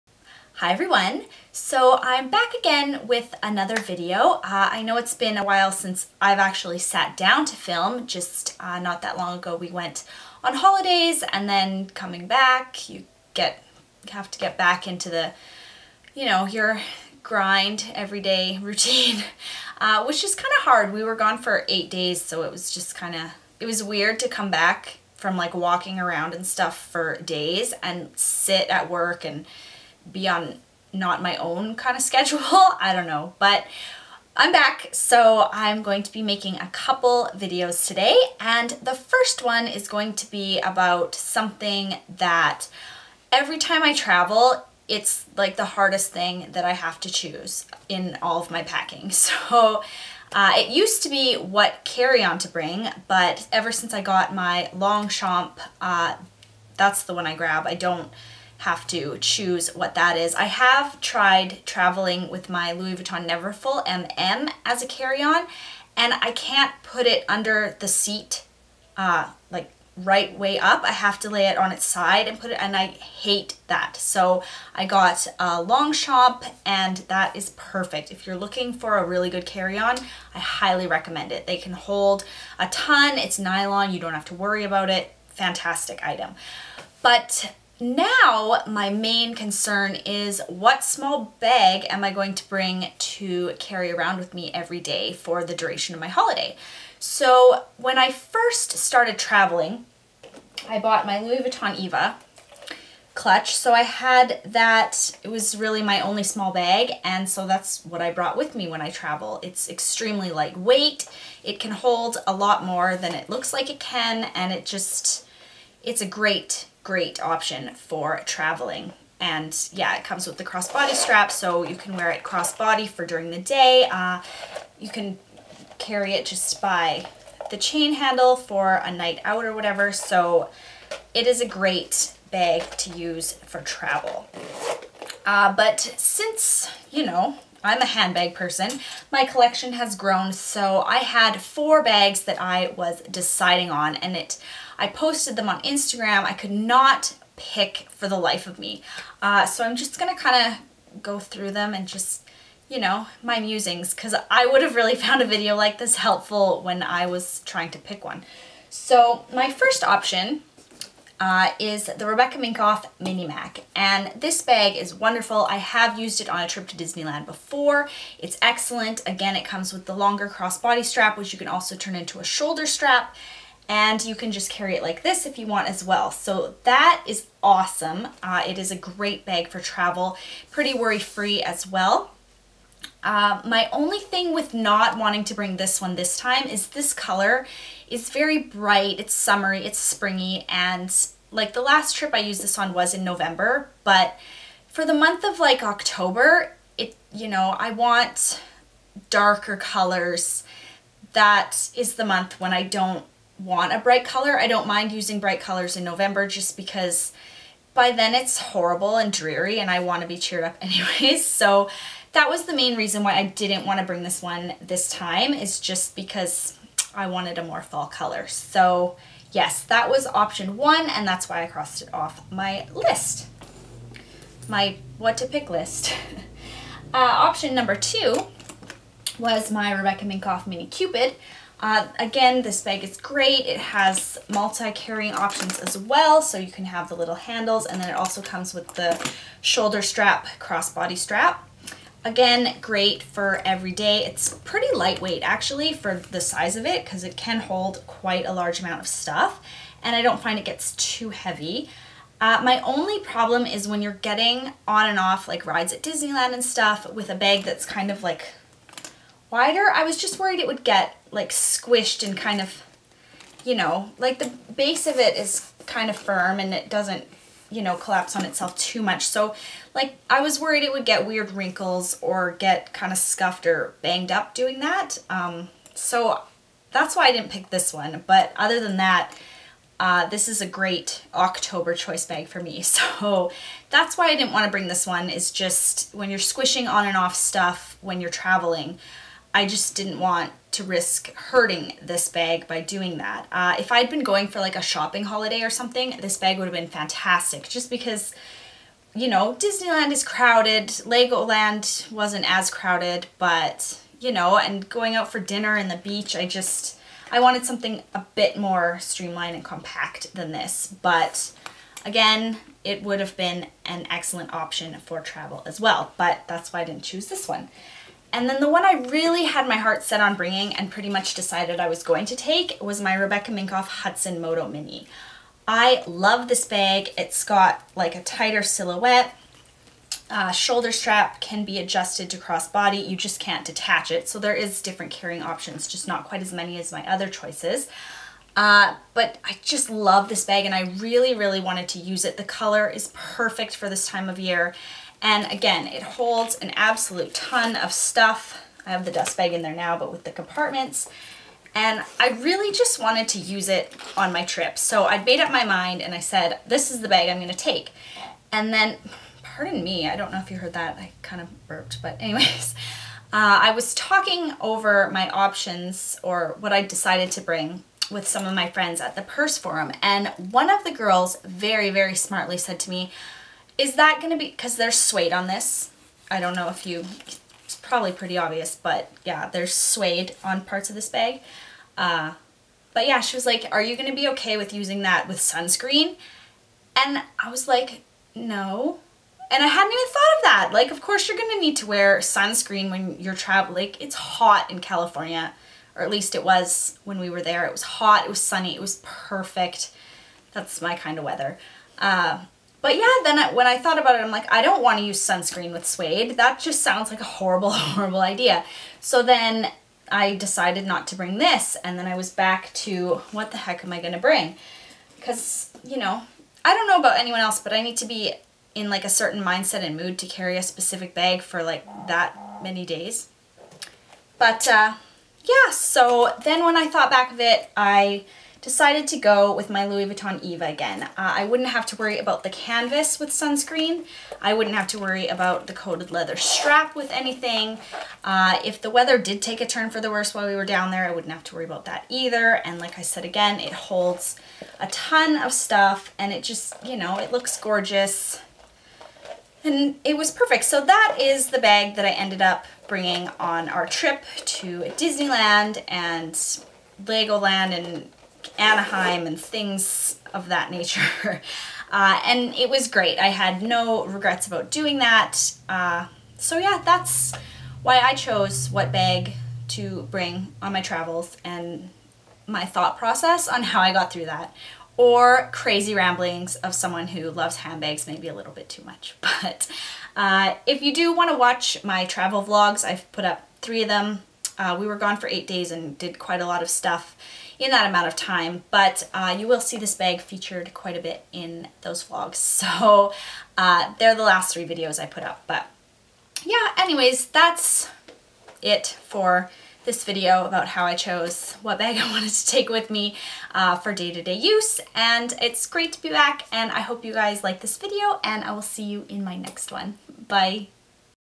Handbag Vlog - Picking a Small Travel Bag
How I decided what small bag to bring with me on my recent trip. This video could also be called, a peak into my bag addled mind when it comes to traveling. Hope you enjoy and as always thank you so much for taking the time to watch!